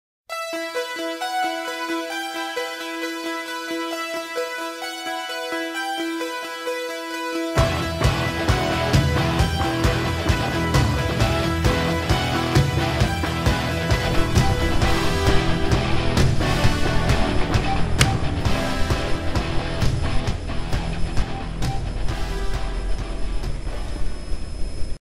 Super Crazy Swing Kick
Kid Gets Knocked Out By Someone On A Swing